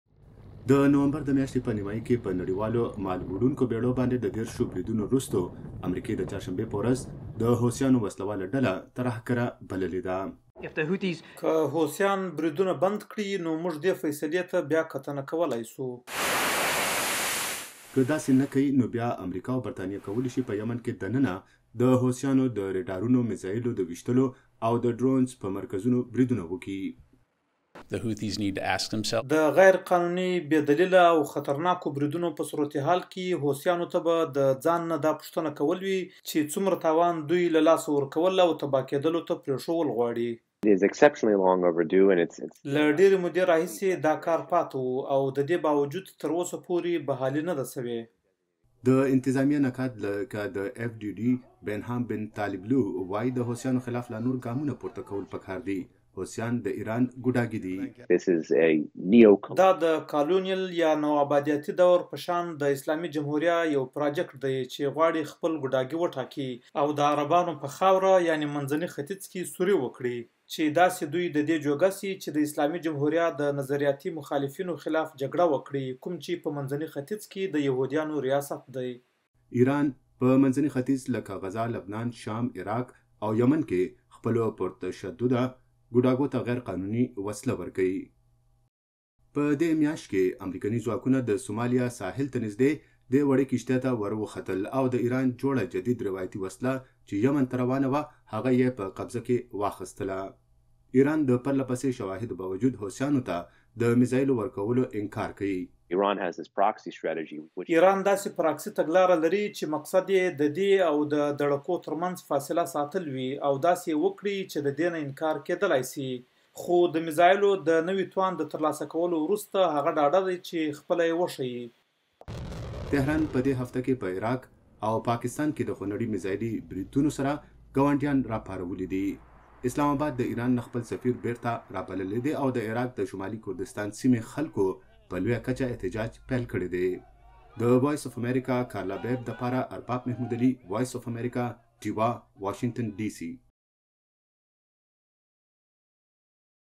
امریکې د یمن حوثي باغیان ترهګر بللي
په بحیره احمر کې په سمندري جهازونو د یمن د حوثي باغیانو د حملو وروسته امریکې حوثیان ترهګر وبللی او ویې وېل که حوثیان حملې ودروي نو واشنګتن په خپله فېصله نظر ثاني کولی شي. د وي او ای خبریالې کارله باب رپورټ ارباب محمودعلي وړاندې کوي